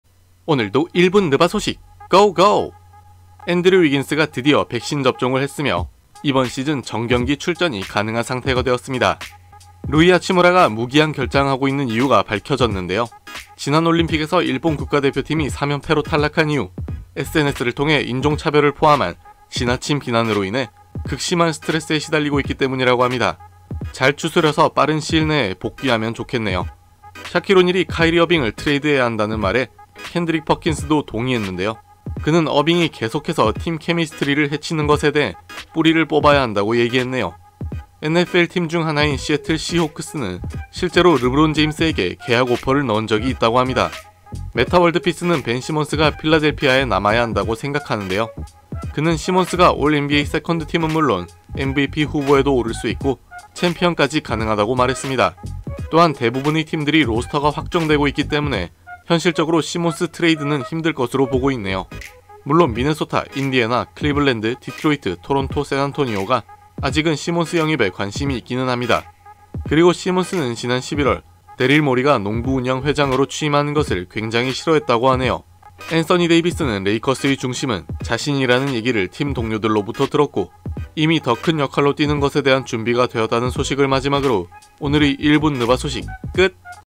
위긴스 백신맞았다!!! 하치무라가 안나오는 이유?! 😨😨😨
하치무라 얼른 돌아와줘... 🙏